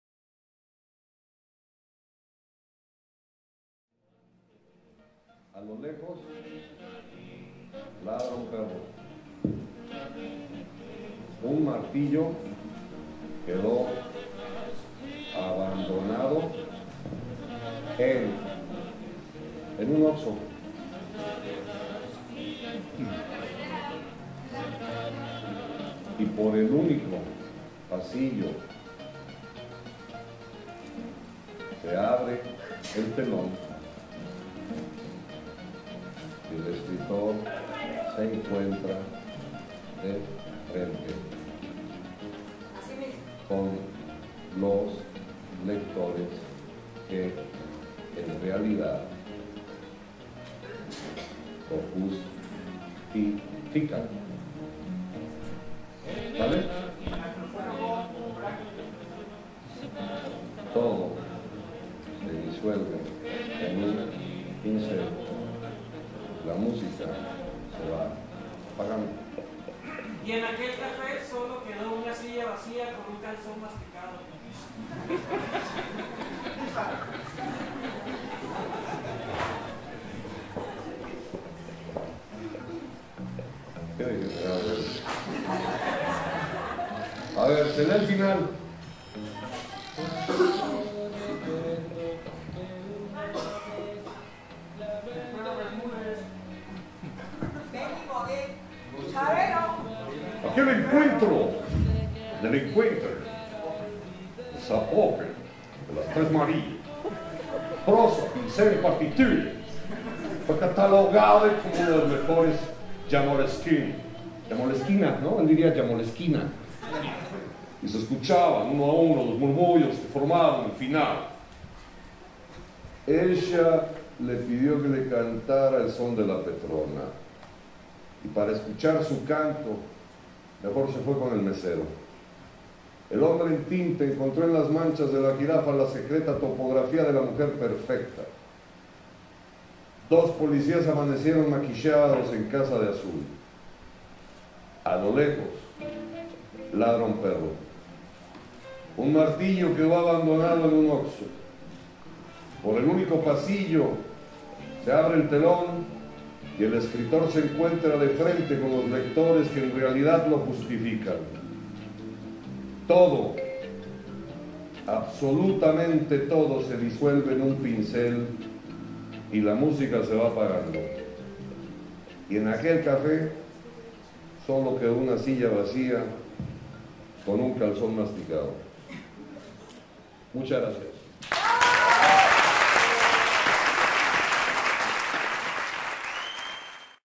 Jam de escritura
El ilustrador Alejandro Magallanes dio vida con sus imágenes a la narración de Jorge F. Hernández, en un jam de escritura organizado por Moleskine, en el Museo Tamayo.